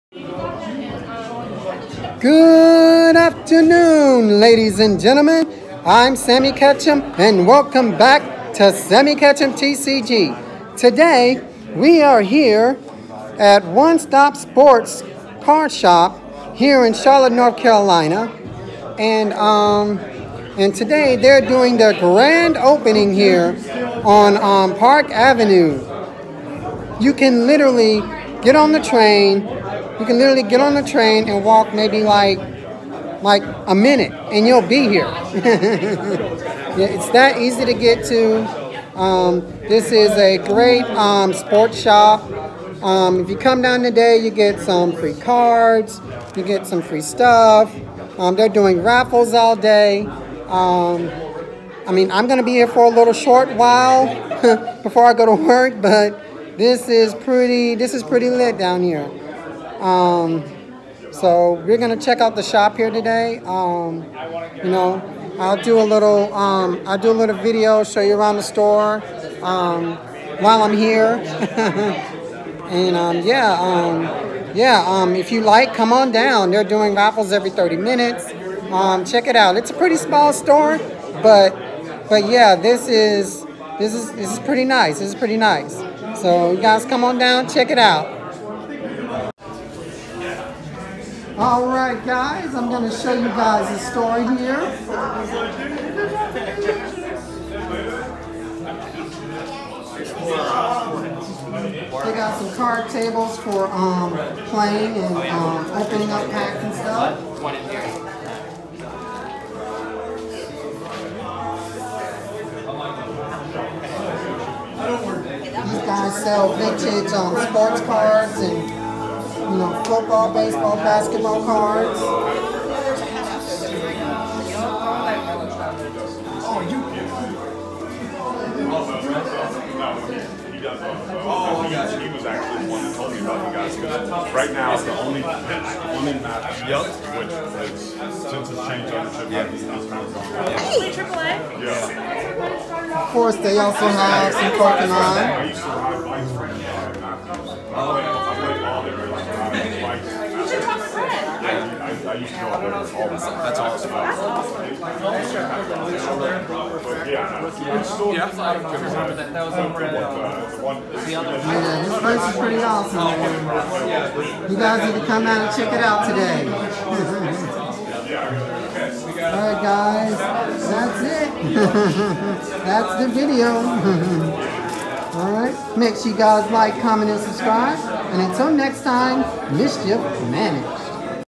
New card shop grand opening!!
As things get better with vaccines,  people, collectors and players are coming back out to go to card/hobby shops. Let’s welcome them back into our lives!!